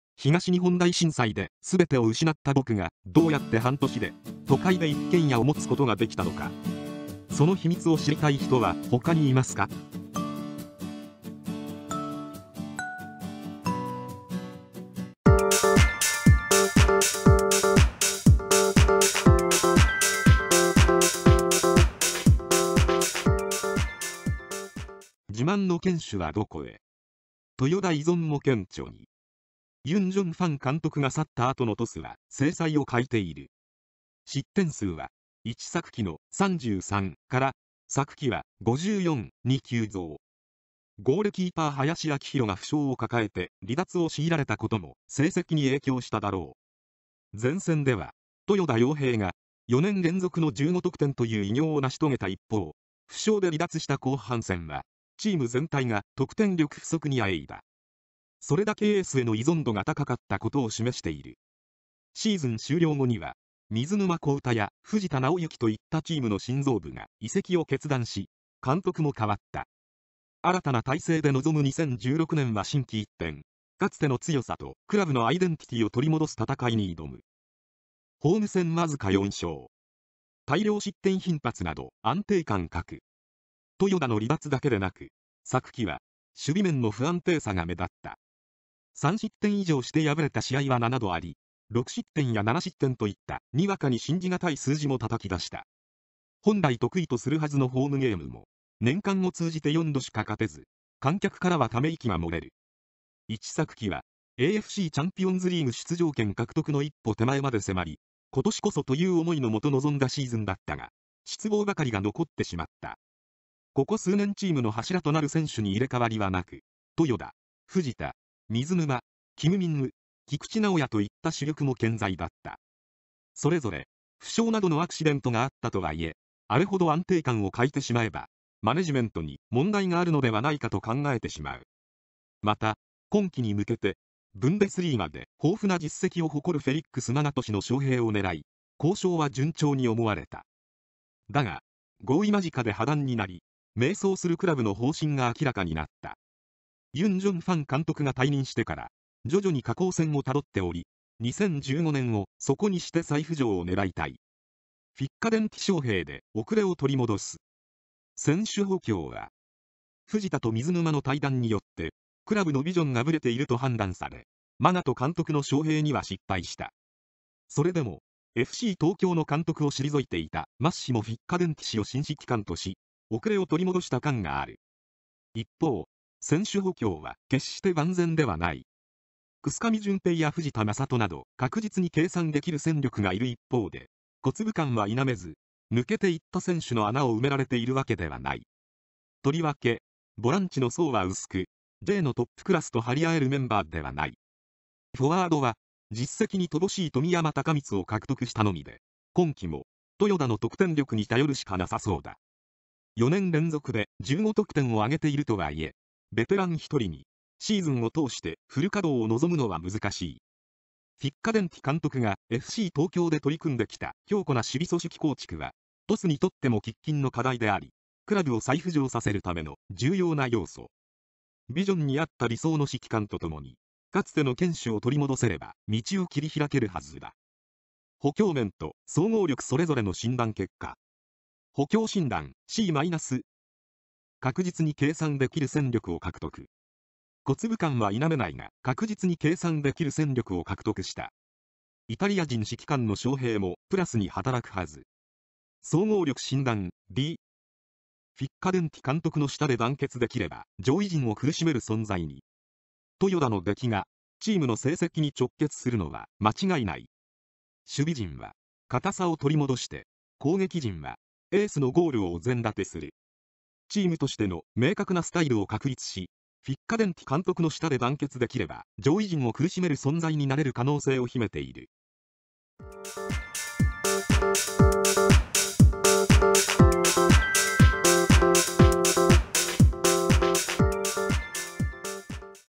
鳥栖、主軸の移籍が大きく響く。フィッカデンティ招聘も豊田依存からの脱却は困難に
Jリーグ開幕に向けて、Jクラブの補強動向を診断していく。今季の目標に向けて、効果的な補強を行うことができたクラブはどこなのか。今回は、新指揮官としてマッシモ・フィッカデンティ氏を招聘したサガン鳥栖を占う。

　前線では豊田陽平が4年連続の15得点という偉業を成し遂げた一方、負傷で離脱した後半戦はチーム全体が得点力不足にあえいだ。それだけエースへの依存度が高かったことを示している。

 　シーズン終了後には水沼宏太や藤田直之といったチームの心臓部が移籍を決断し、監督も変わった。新たな体制で臨む2016年は心機一転、かつての強さとクラブのアイデンティティを取り戻す戦いに挑む。